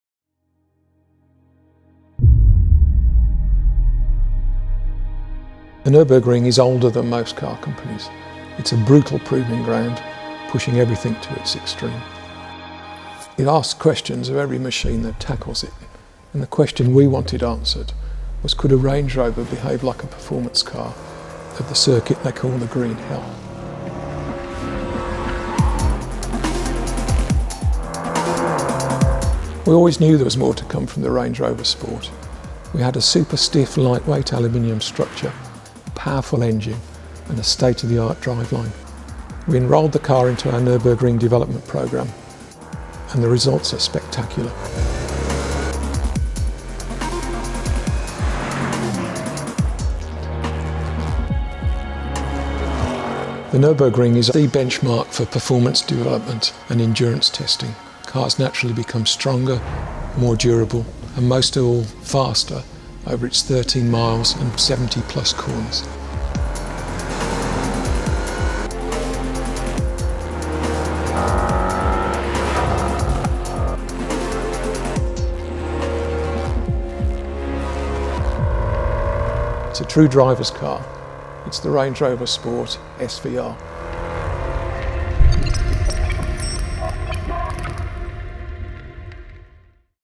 Range Rover Sport SVR - Ultimate Testing at Nürburgring
A true driver's car. The Range Rover Sport SVR unleashes its full potential as it takes on the unforgiving Nürburgring. Known as the 'Green Hell' the Nürburgring is designed to push vehicles to their limit, providing the ultimate challenge for driver and machine.